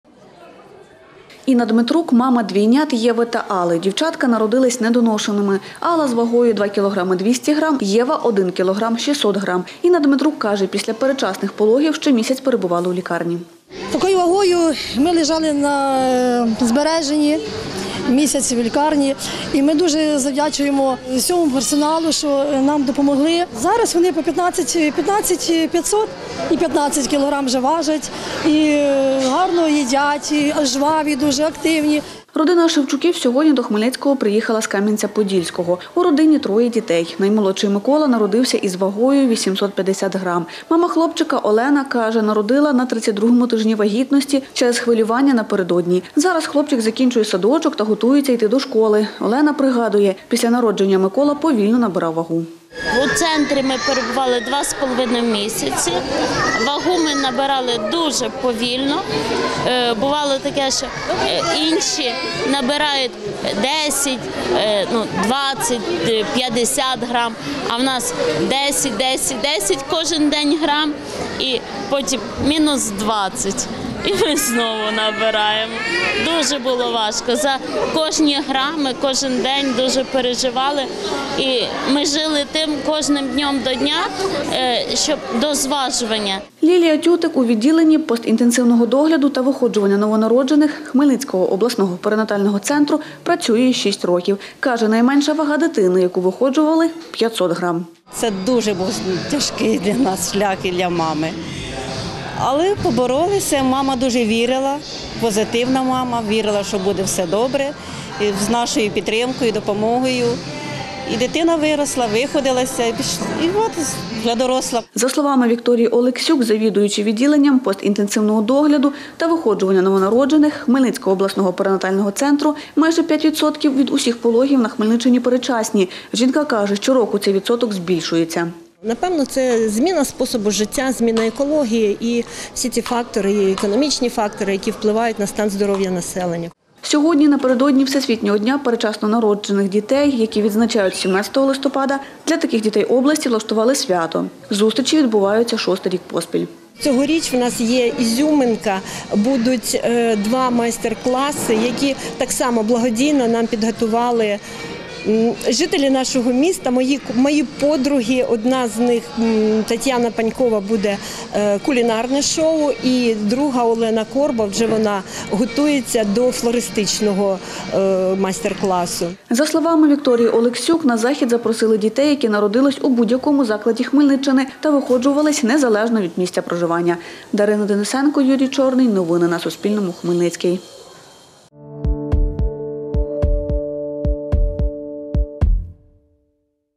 Майже 5 відсотків вагітних Хмельниччини народжують передчасно
Майже 5 відсотків вагітних жительок Хмельниччини, народжують передчасно. Про це розповіла завідуюча відділенням постінтенсивного догляду та виходжування новонароджених Хмельницького обласного перинатального центру Вікторія Олексюк. Жінка каже, це світова тенденція. Сьогодні, напередодні Всесвітнього дня передчасно народжених, у приміщені одного з хмельницьких розважальних закладів для дітей зібралось понад 50 родин області, у яких діти народились передчасно.